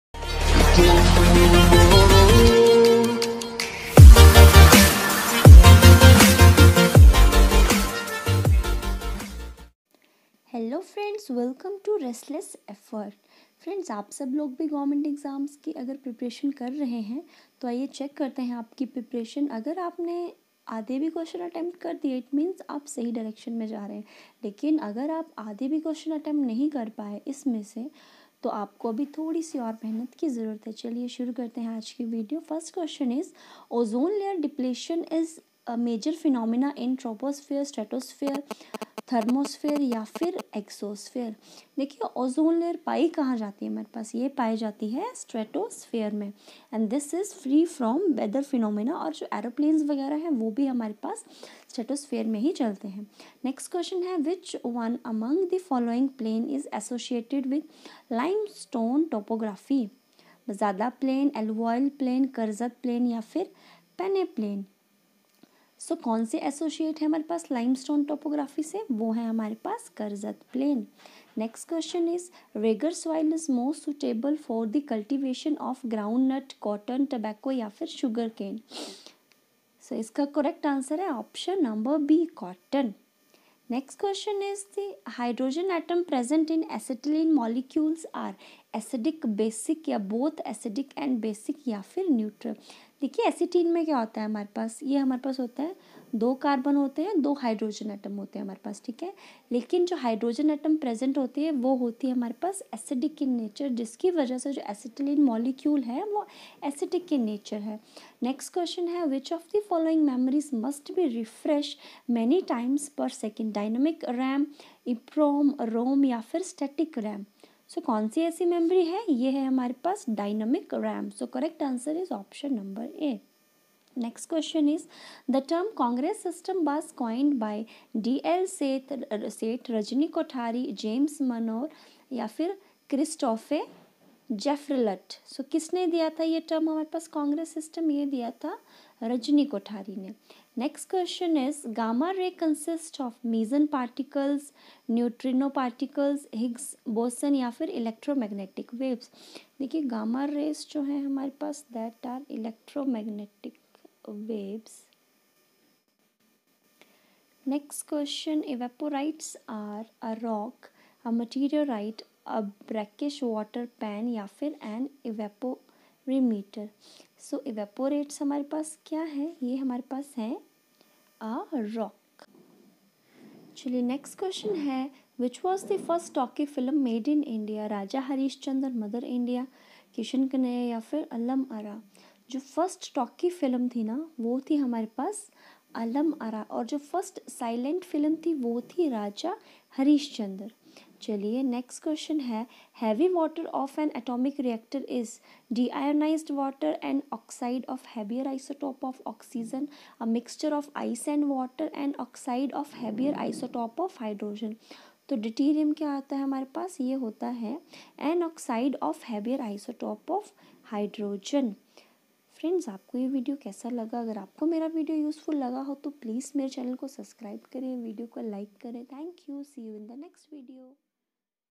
PREVIOUS YEAR QUESTION FOR UPSC/SSC/ RAILWAYS
In this video you will find question from previous year papers from upsc/ssc/railways to check your preparation for upcoming exam